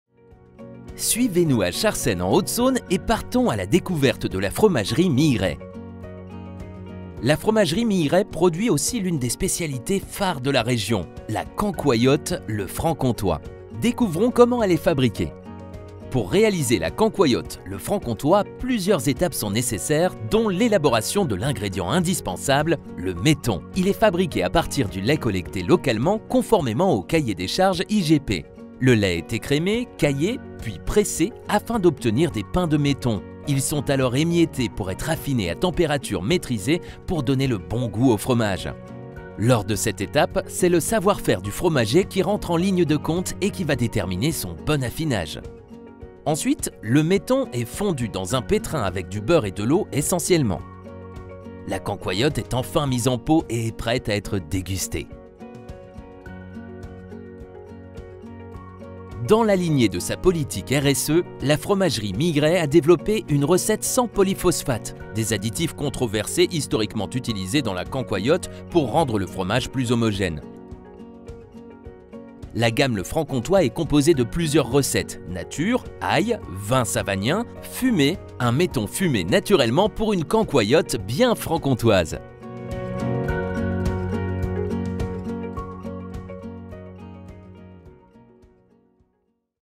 Fromagerie Milleret : La cancoillotte Le Francomtois, ses secrets de fabrication
La Fromagerie Milleret produit aussi l'une des spécialités phares de la région : la Cancoillotte Le Francomtois. Découvrez comment elle est fabriquée.